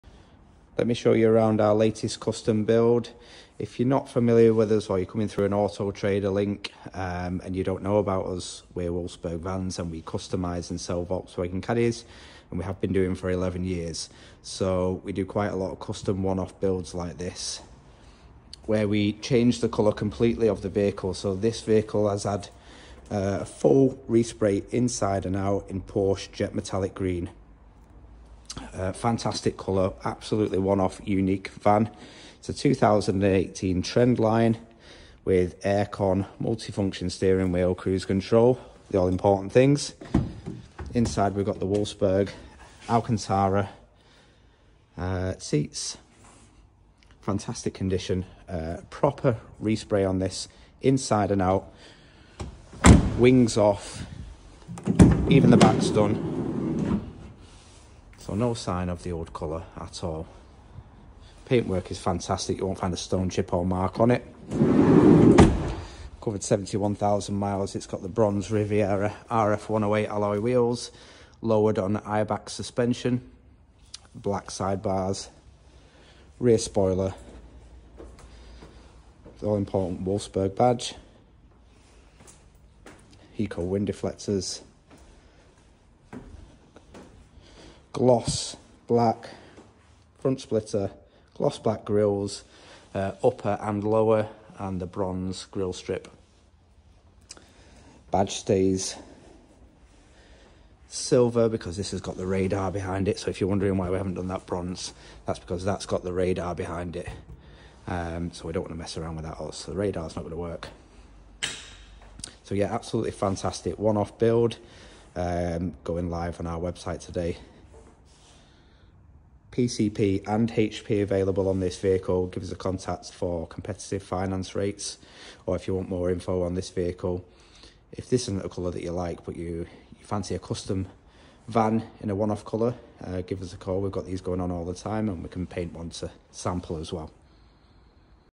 2018 Custom Wolfsburg Caddy in Porsche Jet Metallic Green
Walkaround of this custom one off build, repainted in Porsche Jet Metallic Green